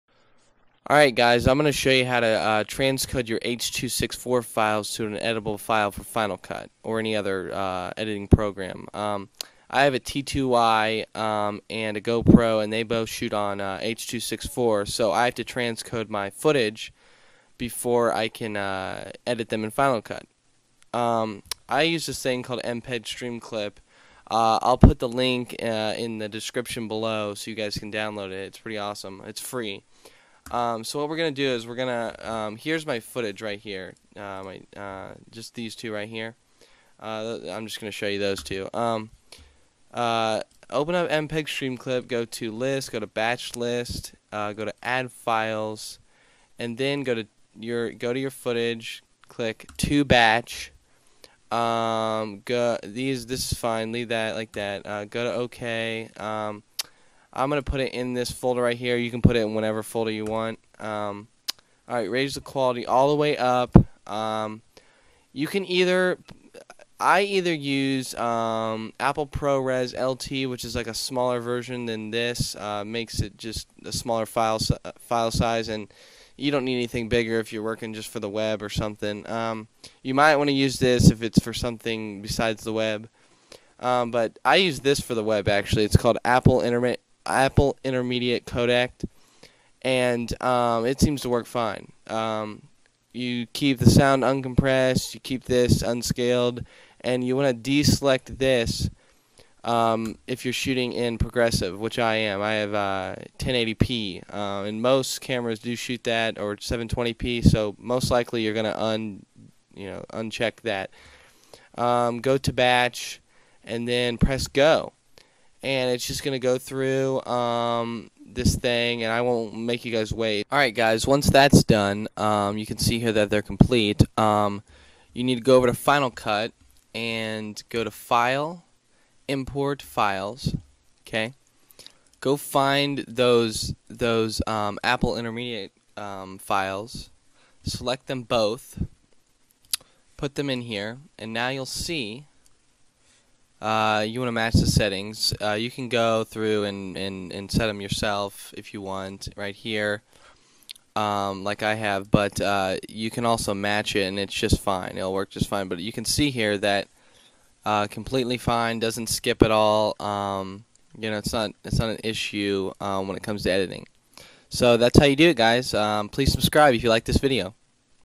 How To Transcode H264 Files To Be Able To Edit In Final Cut Pro
This tutorial goes over how to transcode H264 files to be able to edit them in Final Cut Pro.